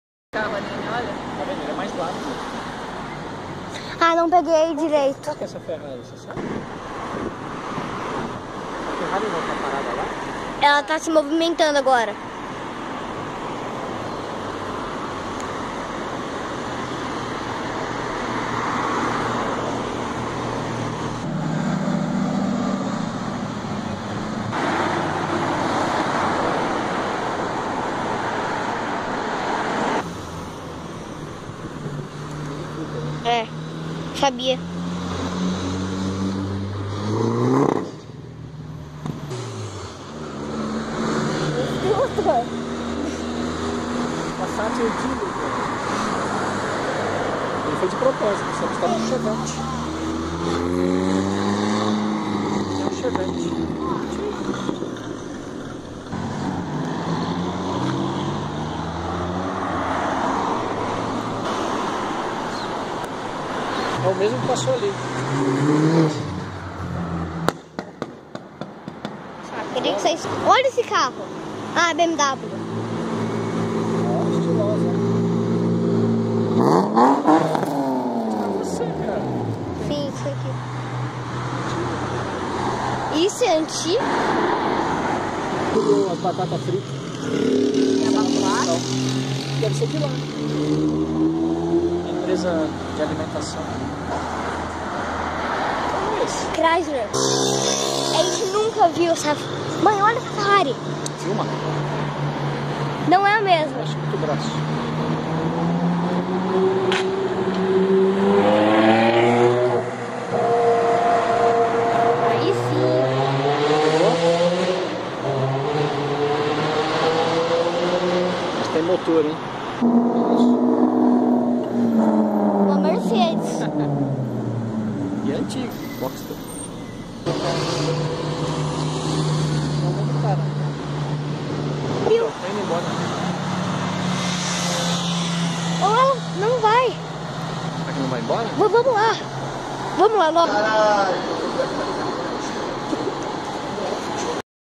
[GUGA TV] - Supercarros nos Embalos De Domingo À Tarde
Supercarros Na Av.Europa, num domingo à tarde qualquer..